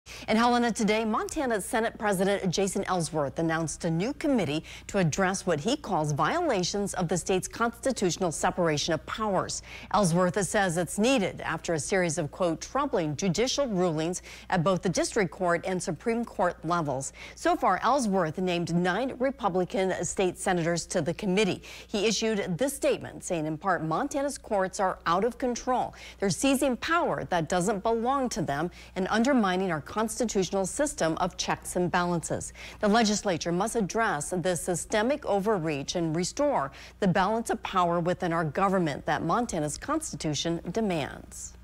Montana Senate president launches committee to address 'separation of powers' violations
Montana Senate President Jason Ellsworth announced he is appointing a new committee to address violations of the state's "separation of powers."

Ellsworth says the Montana Supreme Court and judicial branch violated the state's separation of powers.

He says the committee is needed after a series of "troubling judicial rulings at both the district court and Supreme Court levels."

Ellsworth has named nine Republican state senators to the committee. Additional members are still being decided.